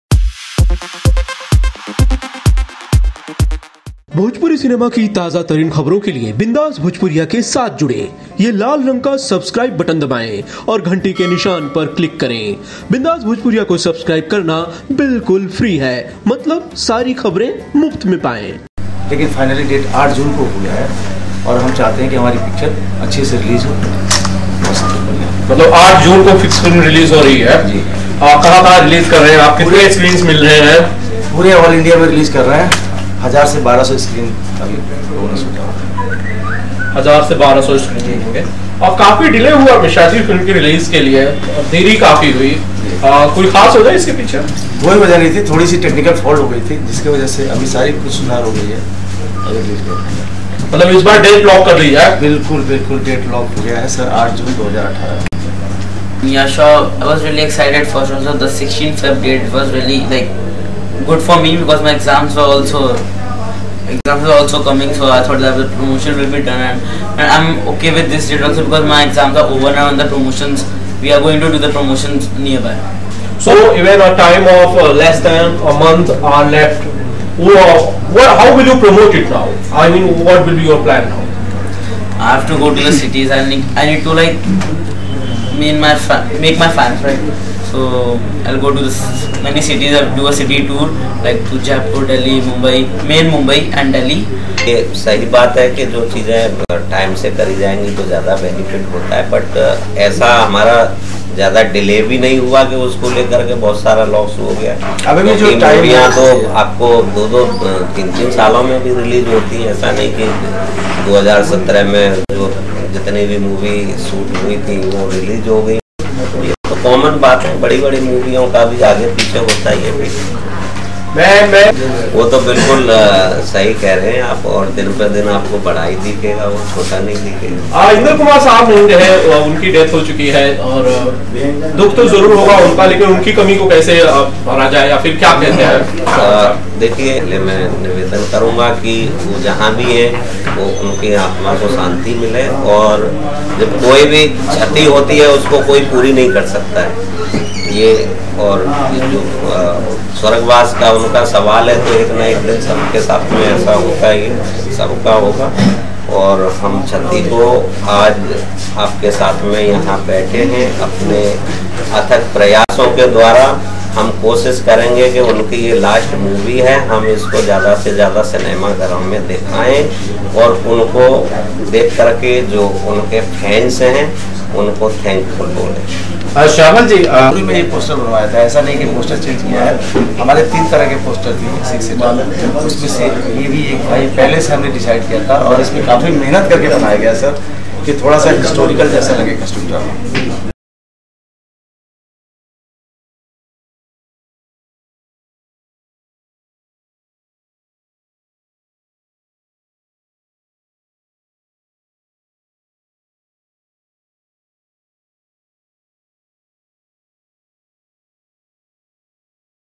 बॉलीवुड एक्टर इंद्र कुमार की मौत के एक साल बाद आखिरी फिल्म होगी रिलीज़ | Bindaas Bhojpuriya
इंद्र कुमार की  आखिरी फिल्म ' क्रीना ' 8 जून को होगी रिलीज़. Late Bollywood actor Inder Kumar's last Hindi movie Krina will release next month.